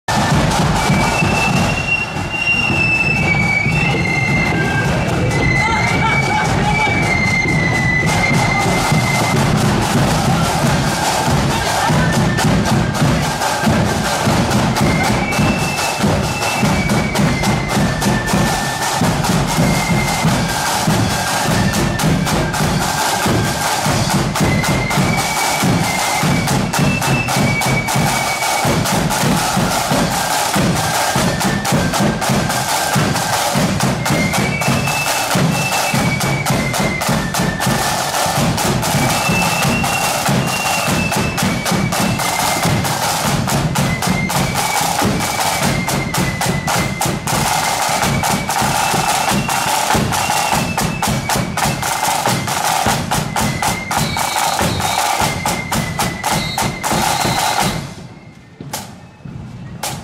Portadown Defenders ( No.1) @ Mid Ulster Mem. Parade 25/09/21 (4K)
Portadown Defenders Flute Band parading through the streets of Portadown at the annual Mid Ulster Memorial parade held in the Co. Armagh town on Saturday 25/09/21.